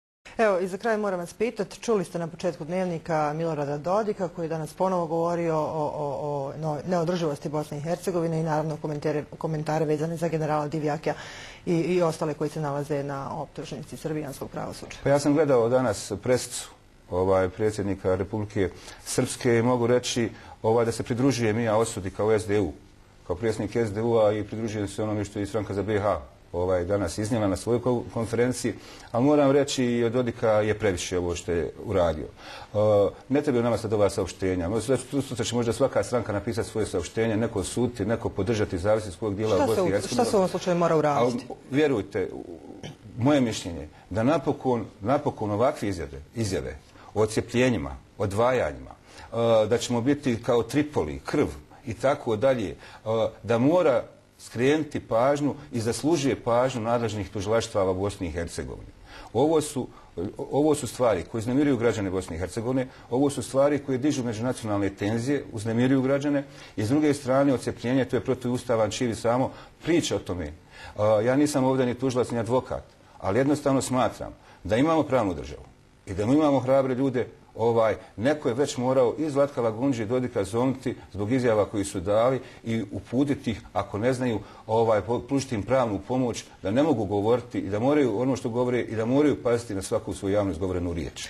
Nermin Pećanac & Dodikovo otcjepljenje (24sata.info)
Nermin Pećanac, predsjednik Socijaldemokratske unije (SDU) BiH, smatra da današnja izjava predsjednika RS o neodrživosti BiH i "ako je i od Dodika, previše je".